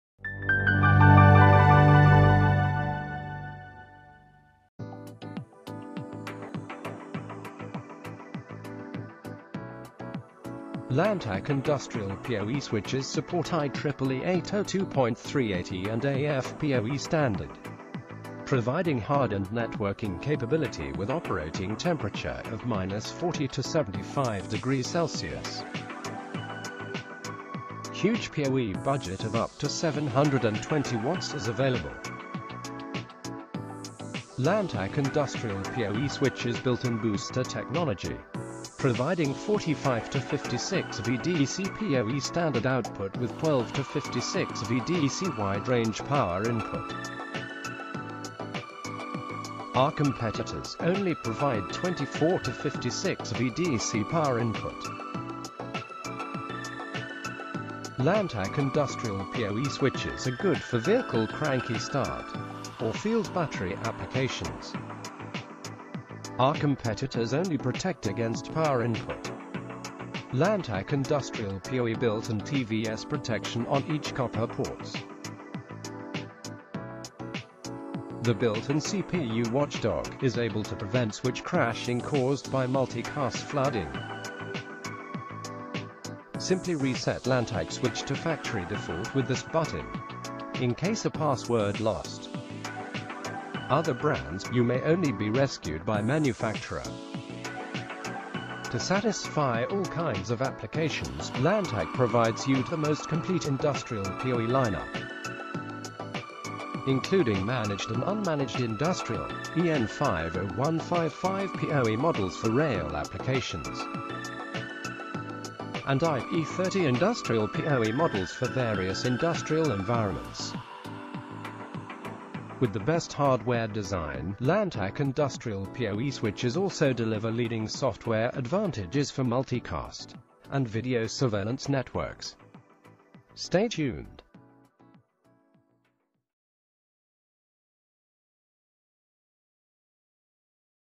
Lantech Industrial PoE - Hardware Advantages
This short clip shows you the hardware advantages of Lantech industrial PoE switches, including:

- PoE at/af support
- Wide operating temperature
- Up to 720W PoE budget
- Built-in booster, 12~56VDC power input
- TVS protection
- CPU watchdog
- Physical reset button
- Complete product line-up